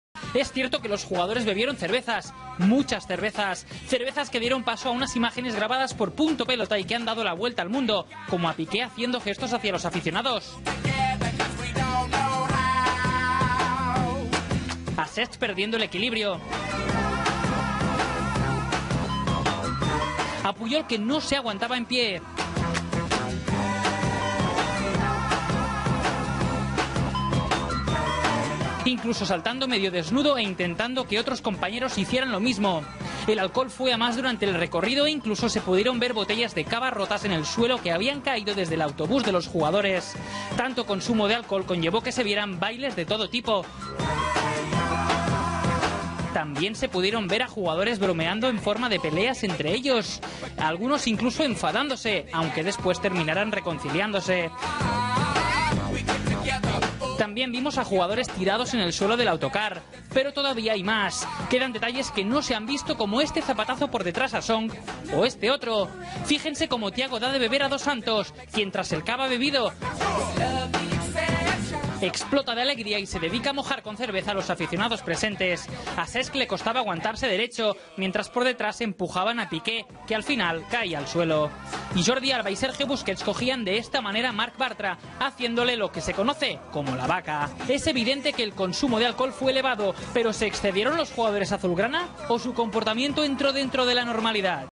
Pain in Spain as Barca team mates clash on bus
Barcelona stars Gerard Pique and Alex Song appear to have been scrapping while on top of the team's double-decker bus to celebrate their La Liga triumph.